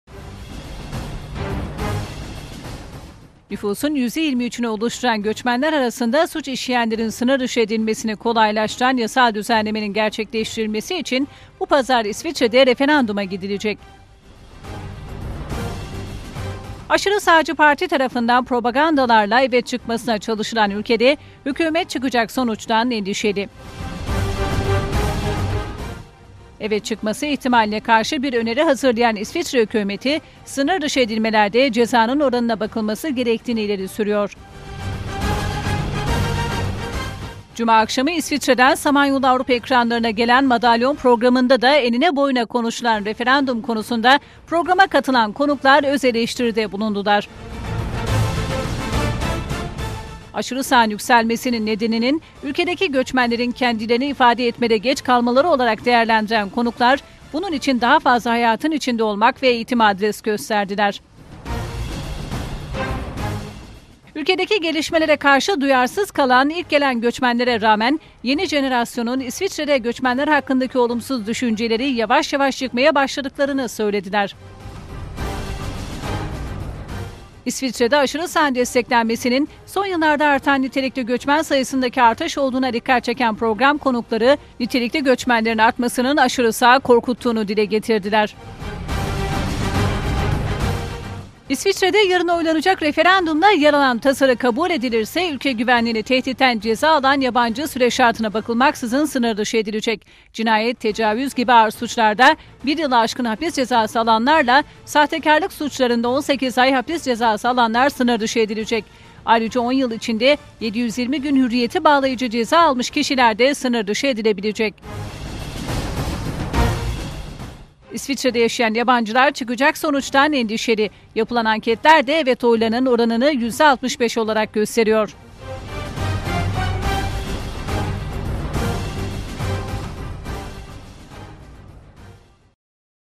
Suç işleyenlere sınırdışı ! 27.11.2010
İsviçre'de referanduam gidilecek
ÜLKEDEKİ SUÇ İŞLEYEN YABANCILARIN SINIRDIŞI EDİLMESİ İSTENİYOR